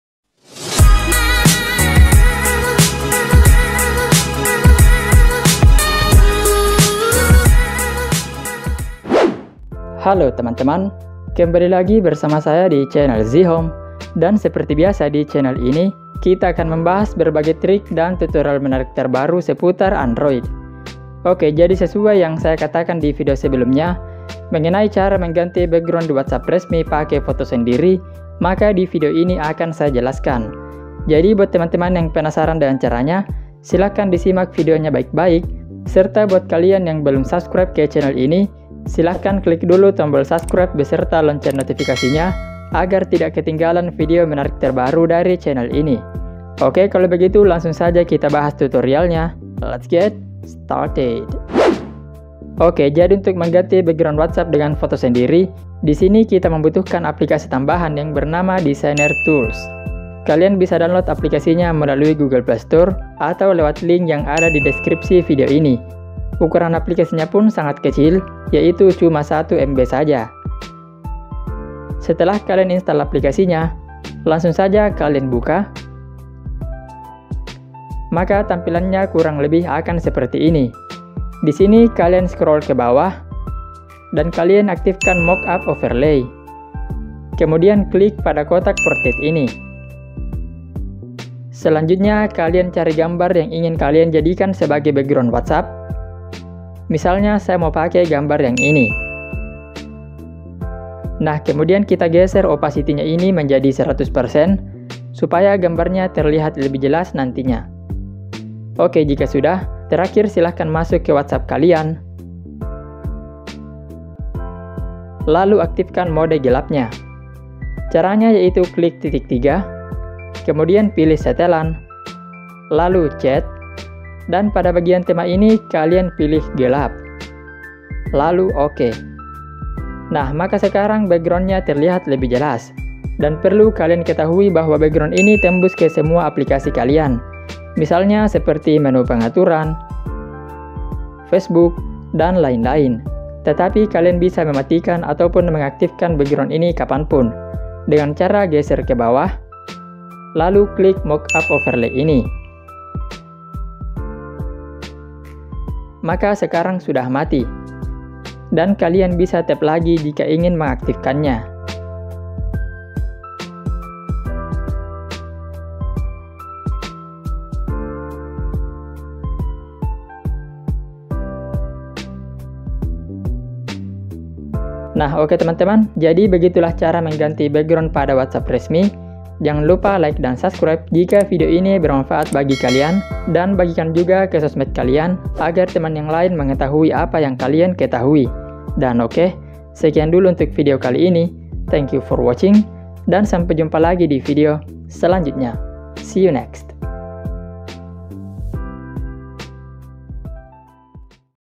Cara Ganti Background di WhatsApp Resmi Pakai Foto Sendiri
Berikut adalah cara ganti background di whatsapp resmi pakai foto sendiri. dimana disini kita menggunakan whatsapp original.
ini adalah trik whatsapp yang bisa kalian pakai untuk membuat tampilan wa jadi lebih keren. karena kalian bisa ganti wallpaper yang berbeda di whatsapp.
Tutorial whatsapp atau cara mengganti backround wa ini bisa di semua android.

Gear yang saya gunakan:

Device        : Vivo v11
Editing        : Wondershare Filmora
Thumbnail : Corel Draw X8